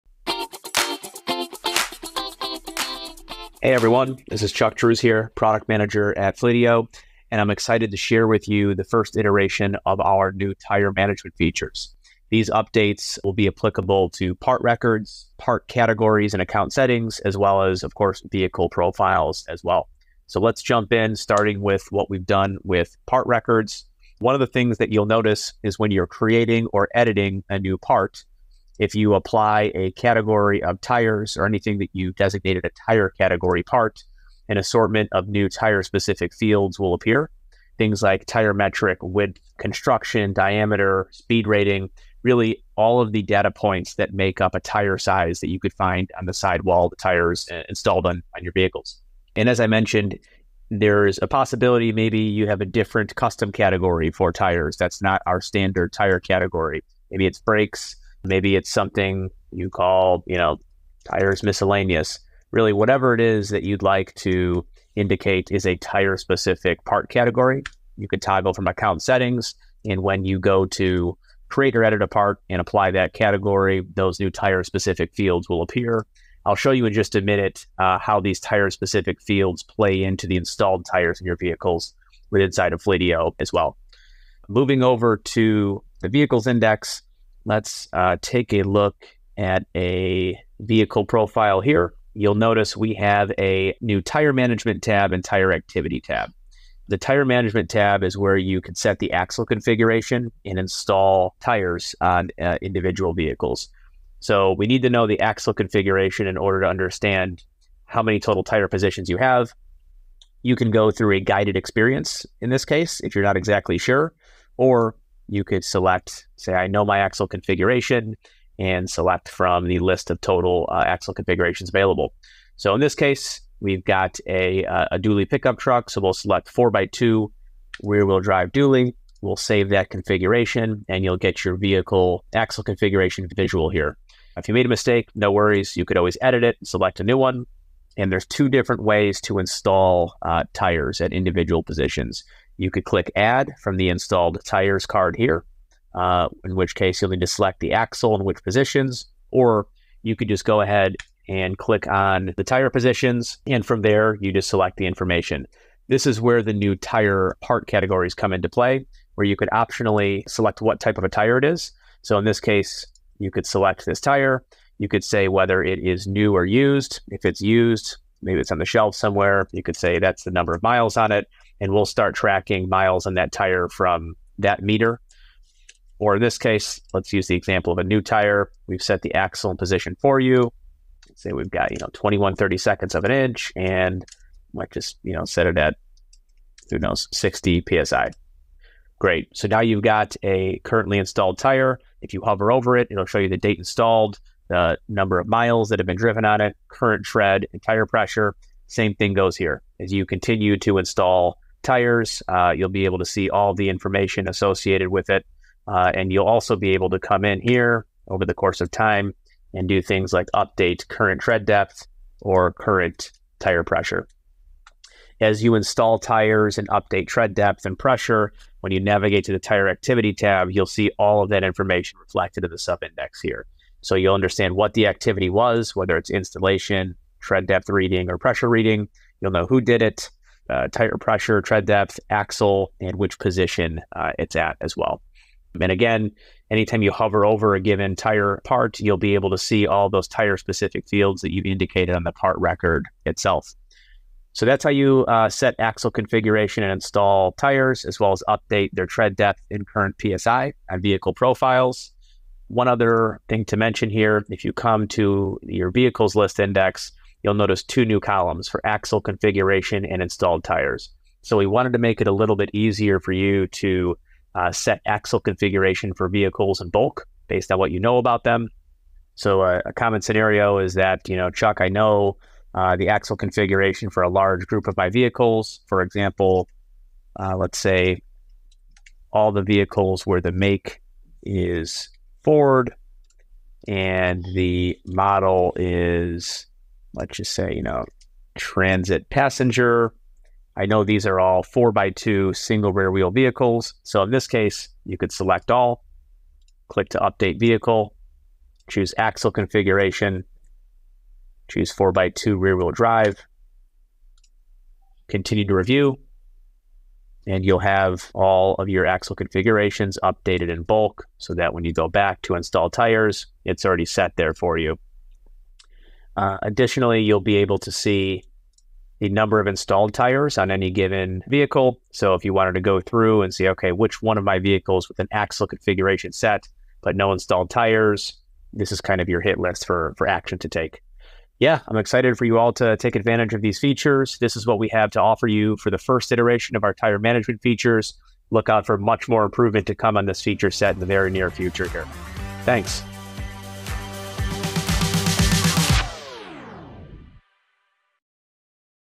New Feature Walkthrough: Fleet Tire Management in Fleetio
Track where your tires are installed and how they’re holding up. Optimize tire maintenance to improve fuel efficiency, enhance safety and reduce operational costs. 🛞

With Fleetio's Tire Management solution, you can:
• Set vehicle axle configurations
• Track all currently installed tires
• Record tread depth and pressure readings

Jump through sections:
00:00 Tire Management in Fleetio
00:28 Part Records & Categorization
01:24 Vehicle Index - New Tire Management & Tire Activity Tabs
04:14 Vehicle Index - New Axle Configuration & Installed Tires Columns
04:30 Vehicle Index - New Axle Configuration
05:22 Vehicle Index - Installed Tires